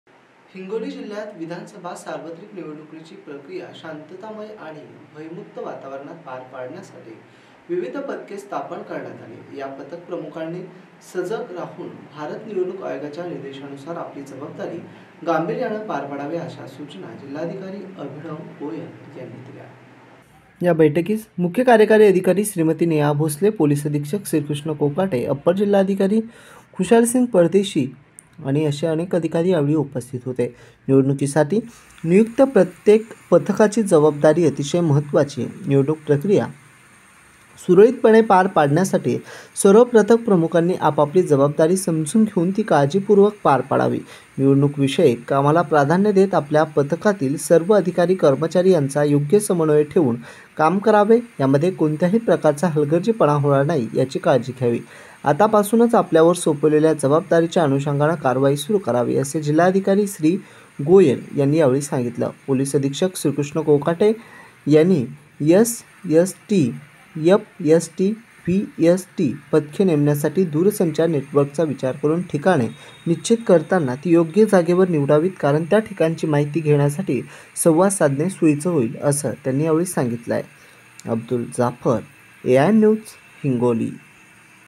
पथक प्रमुखांनी आपली जबाबदारी गांभीर्याने पार पाडावी - हिंगोली जिल्हाधिकारी अभिनव गोयल
विधानसभा निवडणुकीसाठी नियुक्त पथक प्रमुखांनी आपली जबाबदारी गांभीर्याने पार पाडावी - हिंगोली जिल्हाधिकारी अभिनव गोयल

 निवडणूक पथक प्रमुखांचा जिल्हाधिकाऱ्यांनी घेतला आढावा

हिंगोली जिल्ह्यात विधानसभा सार्वत्रिक निवडणुकीची प्रक्रिया शांततामय आणि भयमुक्त वातावरणात पार पाडण्यासाठी विविध पथके स्थापन करण्यात आली आहेत. या पथक प्रमुखांनी सजग राहून भारत निवडणूक आयोगाच्या निर्देशानुसार आपली जबाबदारी गांभीर्याने पार पाडावी, अशा सूचना जिल्हाधिकारी अभिनव गोयल यांनी आज येथे दिल्या. जिल्हाधिकारी कार्यालयात आयोजित निवडणूक पथक प्रमुखांच्या बैठकीत ते बोलत होते.